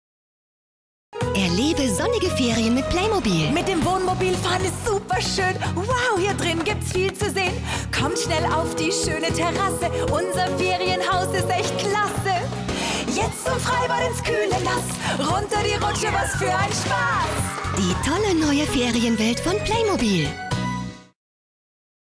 Linha Férias playmobil
Seja em um passeio de lancha, na casa de verão ou passeando com o trailer da família, a diversão ficará completa com a linha playmobil férias